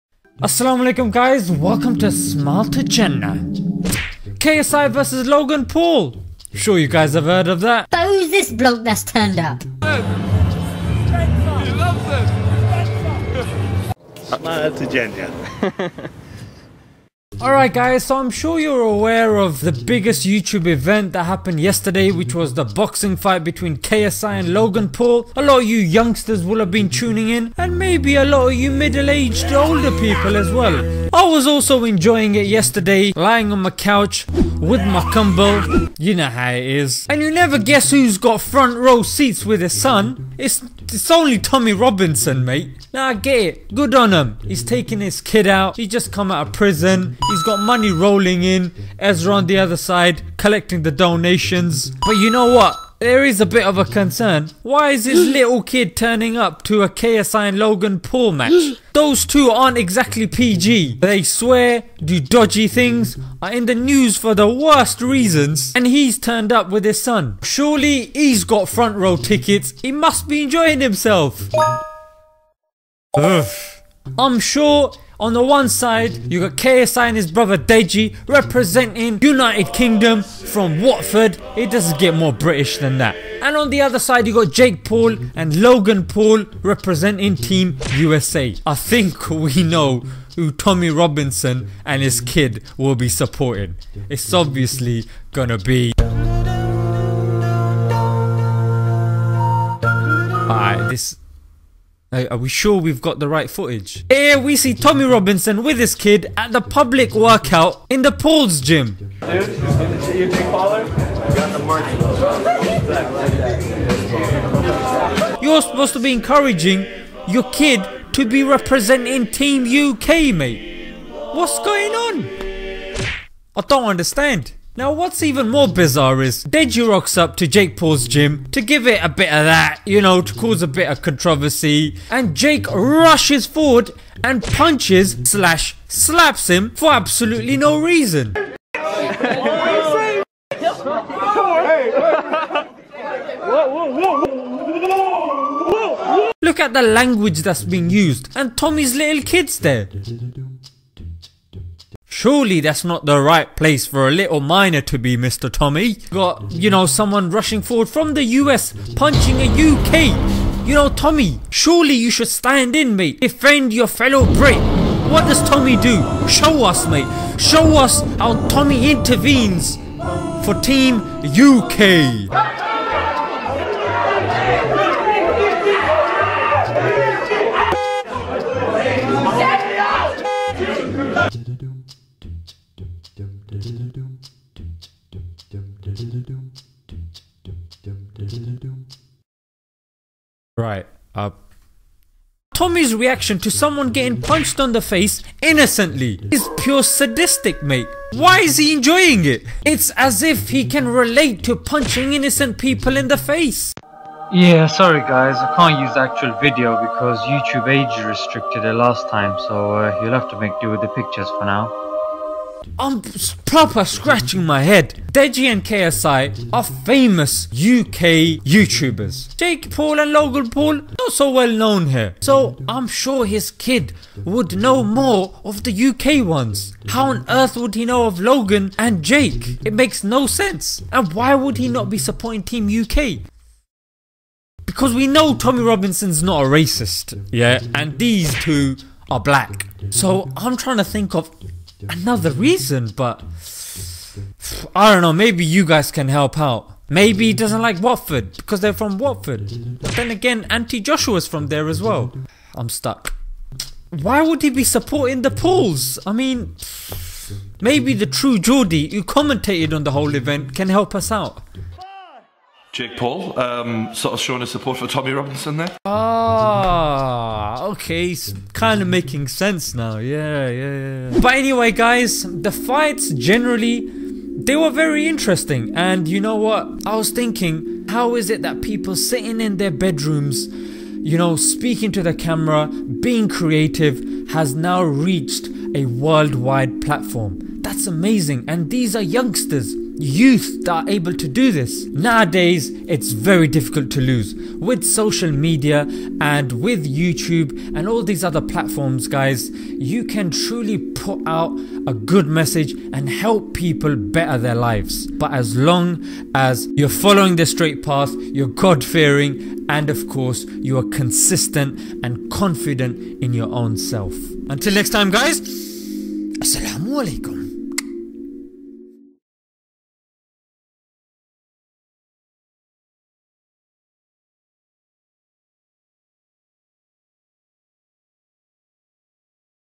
TOMMY ROBINSON AT LOGAN PAUL VS KSI FIGHT
I did not expect to see Tommy Robinson at the KSI vs Logan Paul fight...smh...